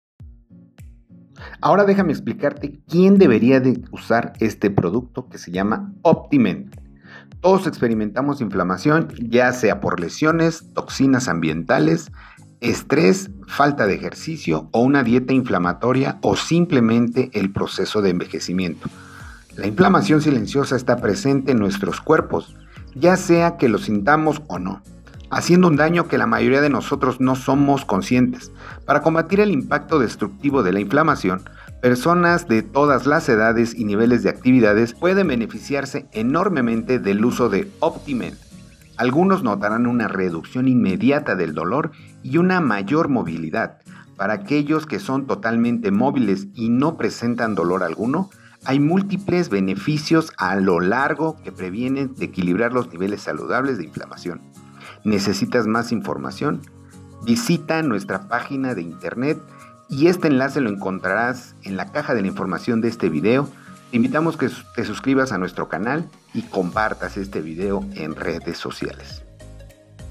🔥🔥🔥 ALERTA Optimend capsulas antes USARLO mira esto!
Optimend es la solución de ACTIVZ para el dolor y la inflamación no saludable. La inflamación saludable es parte del sistema de defensa natural del cuerpo y está diseñada para combatir virus, bacterias y lesiones. Es la forma del cuerpo de "purgarse con fuego". Desafortunadamente, la inflamación excesiva puede causar un aluvión de efectos secundarios desfavorables como calor / fiebre, hinchazón y dolor que pueden dañar los tejidos del cuerpo. Peor aún, la inflamación crónica o "silenciosa" ahora se considera una de las principales causas de la mayoría de las afecciones y enfermedades crónicas que padecemos.